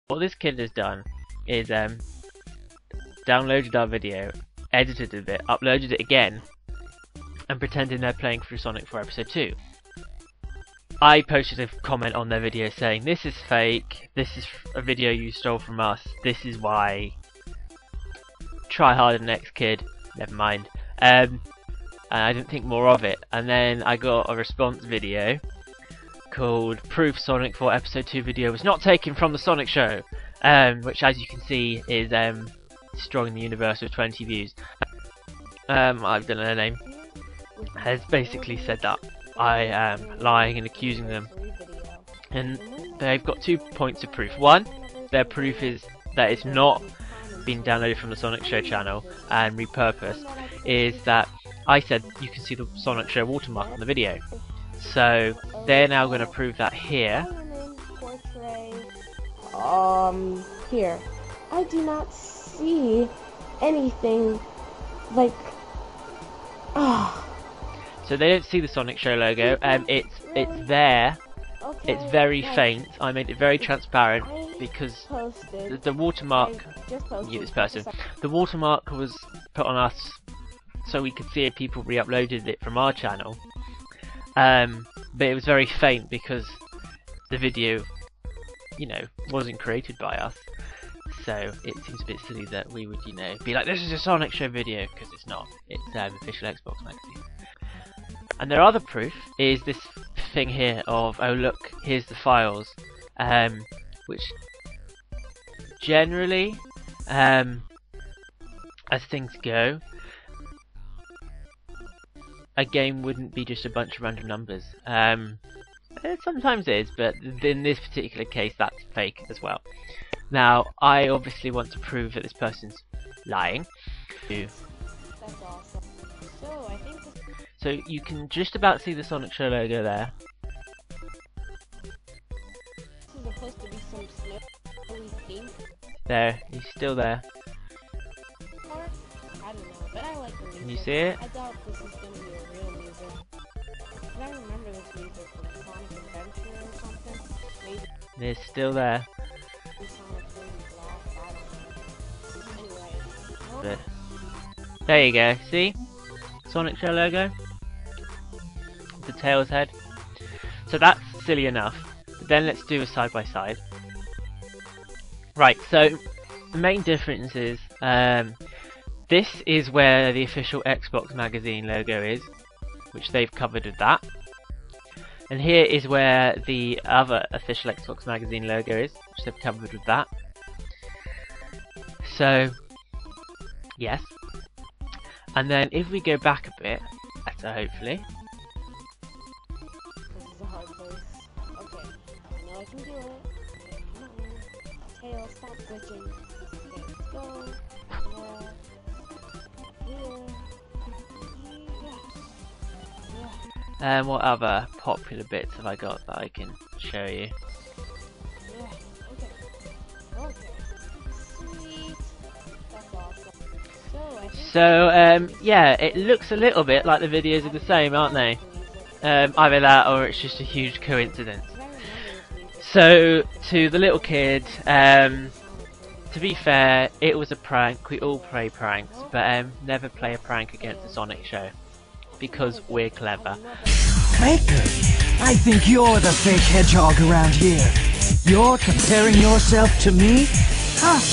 RE: Kid who used our video...
The Sonic Show is fine with people using our channel to get trailers and gameplay footage, but when you pretend it's something else and accuse us of LYING. oh gurl you gonna get owned.